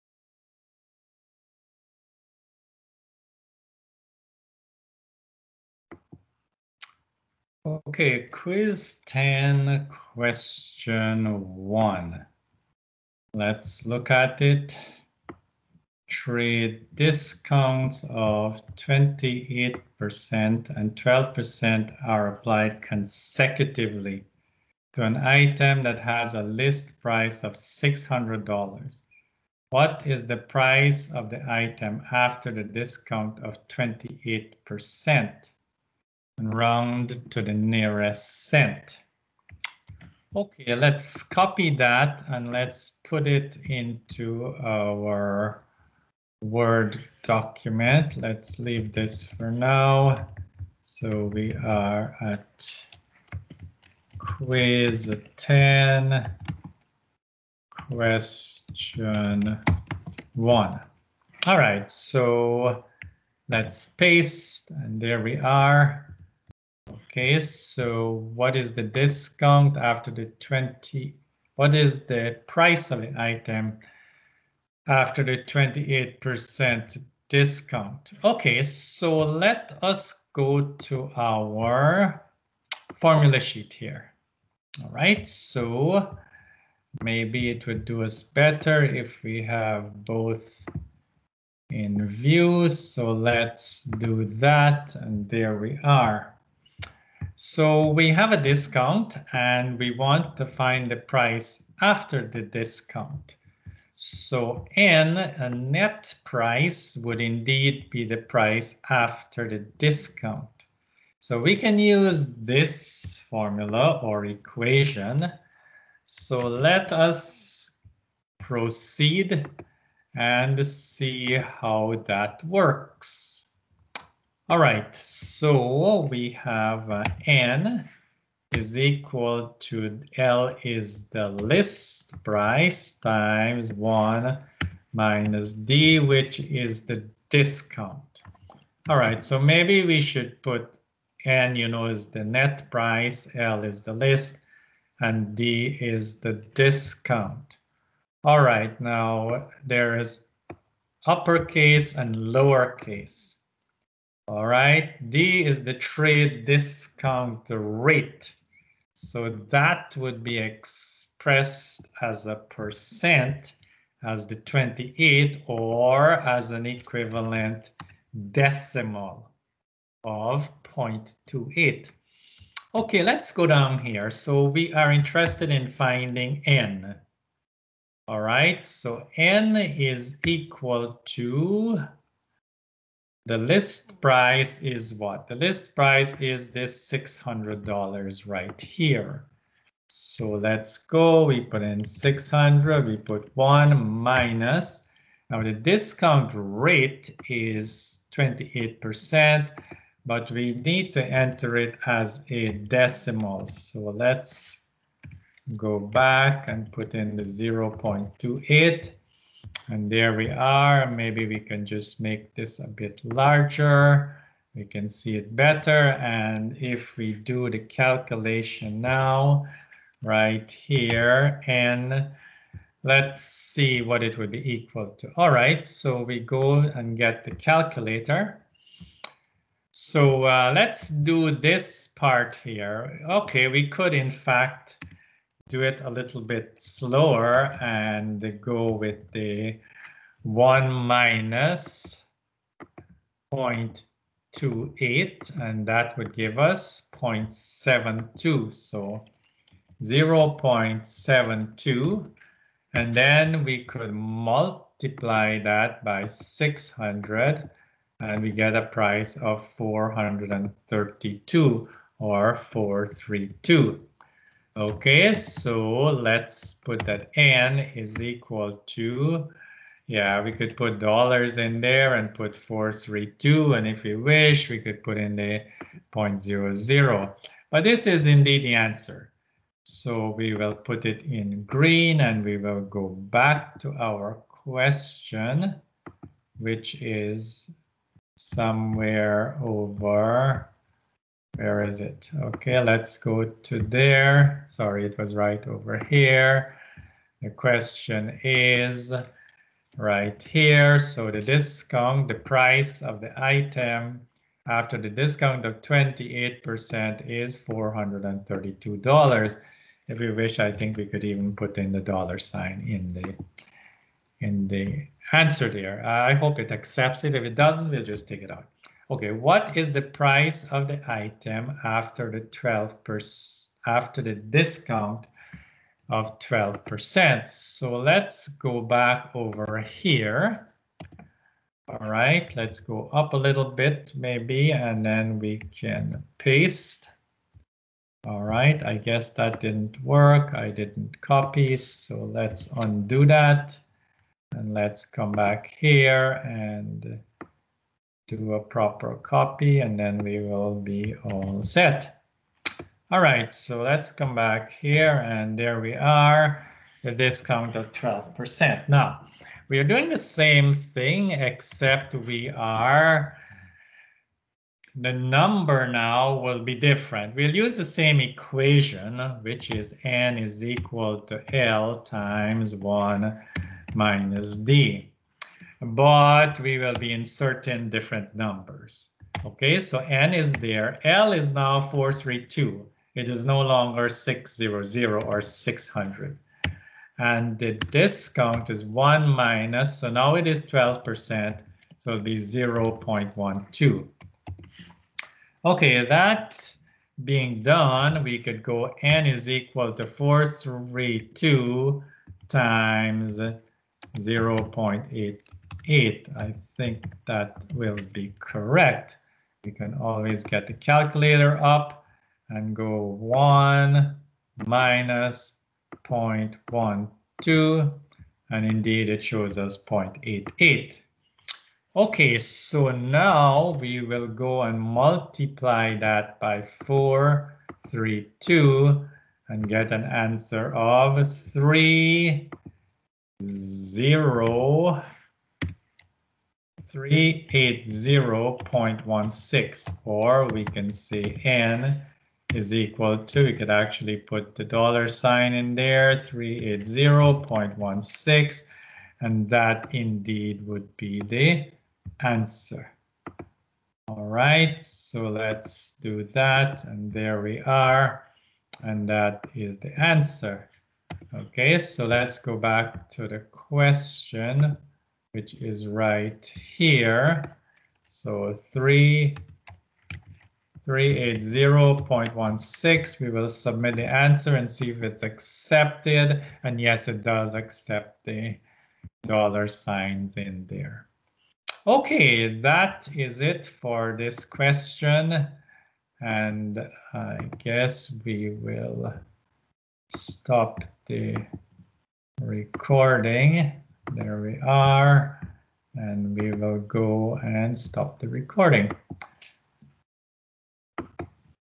Quiz 10, question 2
Trade discounts of 28% and12% applied consecutively.